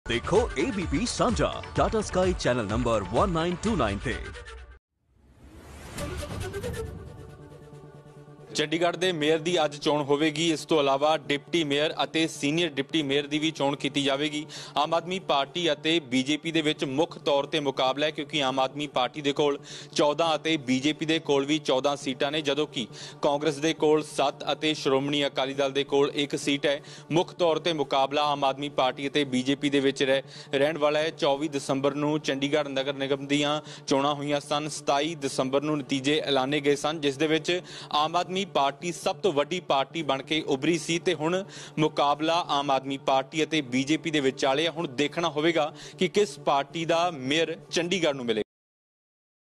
Chandigarh Mayor Election : ਚੰਡੀਗੜ ਮੇਅਰ ਦੀ ਅੱਜ ਹੋਵੇਗੀ ਚੋਣ, AAP ਤੇ BJP 'ਚ ਮੁੱਖ ਮੁਕਾਬਲਾ | Abp sanjha
ਚੰਡੀਗੜ੍ਹ ਨਗਰ ਨਿਗਮ ਦੇ ਮੇਅਰ ਦੀ ਹੋਵੇਗੀ ਚੋਣ
AAP ਤੇ BJP 'ਚ ਮੁੱਖ ਤੌਰ 'ਤੇ ਮੁਕਾਬਲਾ
AAP- 14,  BJP- 14 ਤੇ ਕਾਂਗਰਸ- 7  ਸੀਟਾਂ